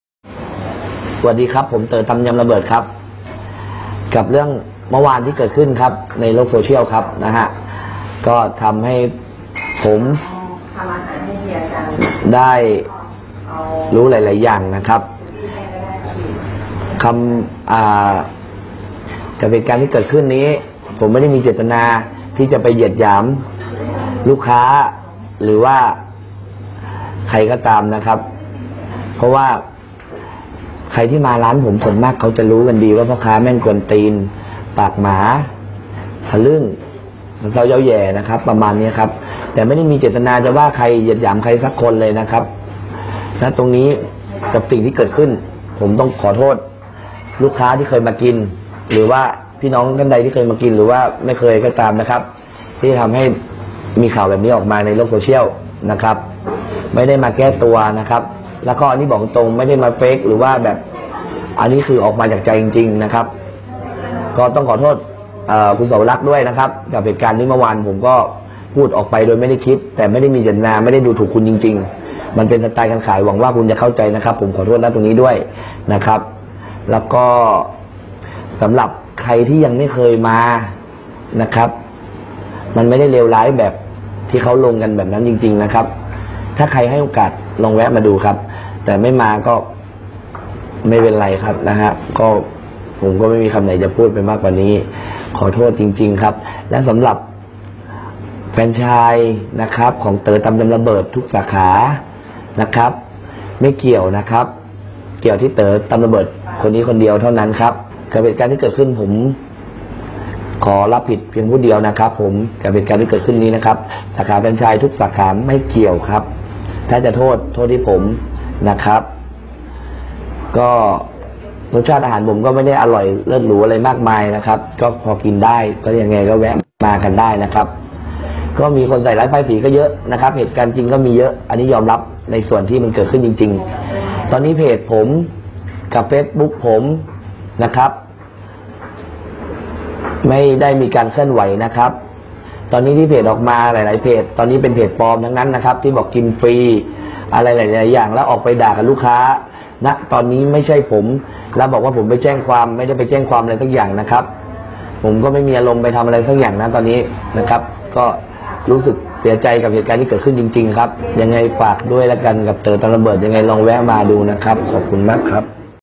ไม่เกินคาด!หนุ่มพ่อค้าส้มตำร้านดังเชียงใหม่อัดคลิปขอโทษลูกค้าสาวแล้วกรณีพูดเหยียด-อ้างไม่ตั้งใจ
24/12/2019 ไม่เกินคาด!หนุ่มพ่อค้าส้มตำร้านดังเชียงใหม่อัดคลิปขอโทษลูกค้าสาวแล้วกรณีพูดเหยียด-อ้างไม่ตั้งใจ

ศูนย์ข่าวเชียงใหม่-พ่อค้าหนุ่มเจ้าของร้านส้มตำดังเชียงใหม่ อัดคลิปขอโทษแล้ว หลังโซเชียลรุมด่า กรณีลูกค้าสาวถูกพูดจาไม่ดีส่อเสียดเหยียดหยามหยาบคาย จนเอาไปโพสต์ตำหนิในโซเชียล อ้างไม่ได้ตั้งใจพร้อมยอมรับได้รับบทเรียนแล้ว